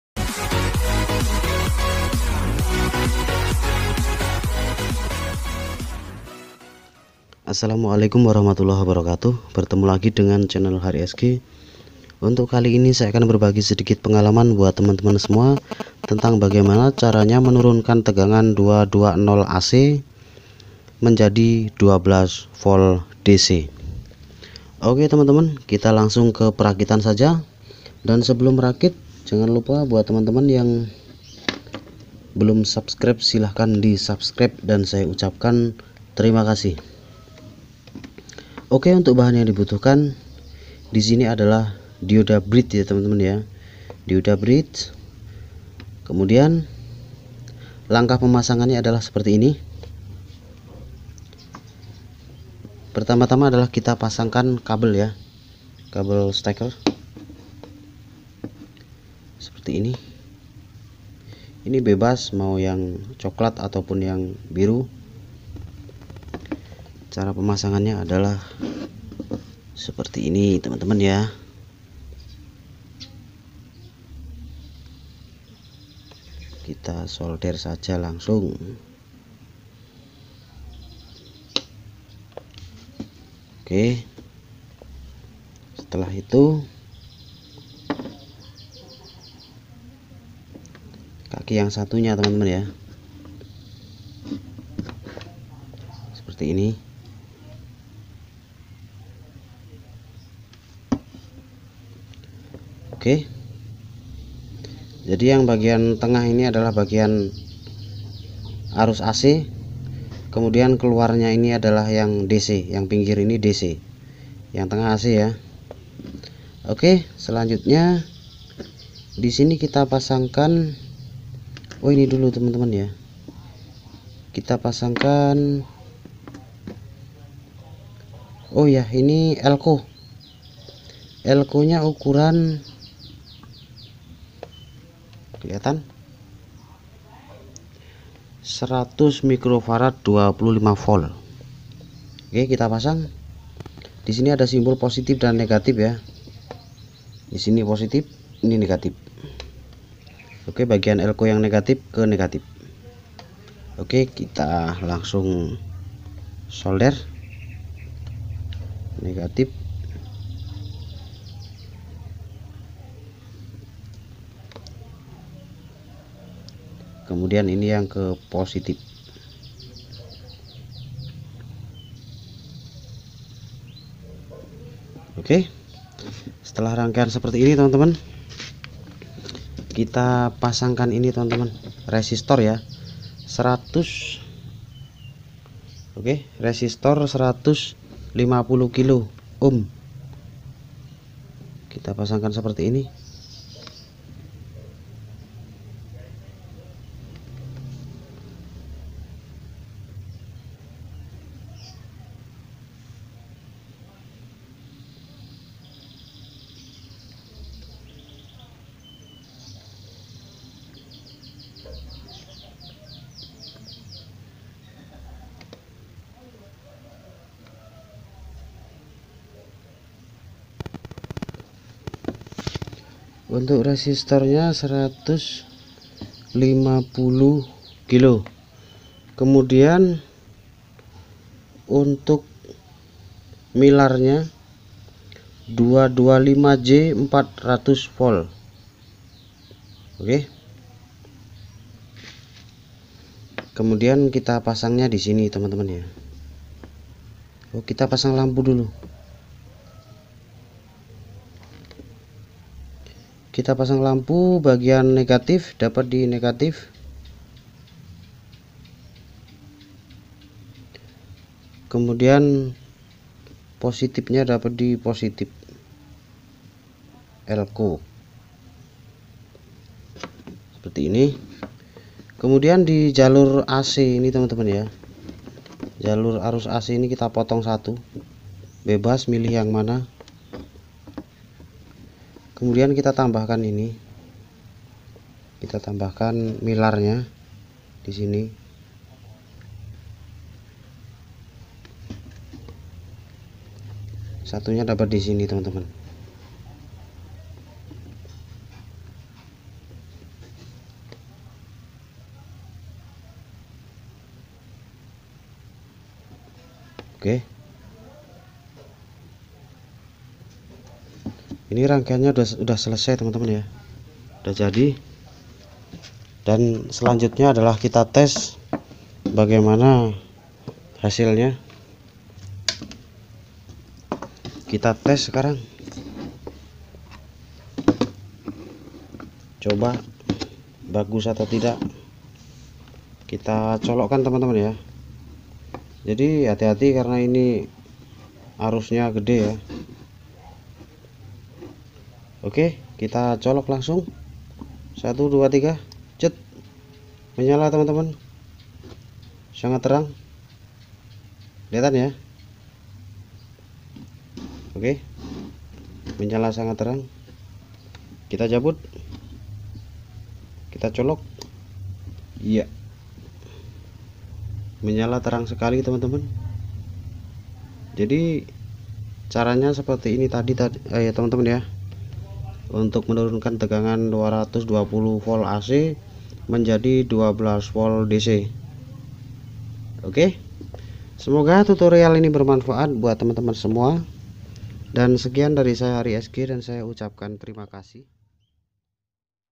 220 v jadi 12v tanpa trafo
output rangkaian ini mantab berada di 12volt..
semoga bermanfaat..
terimakasih...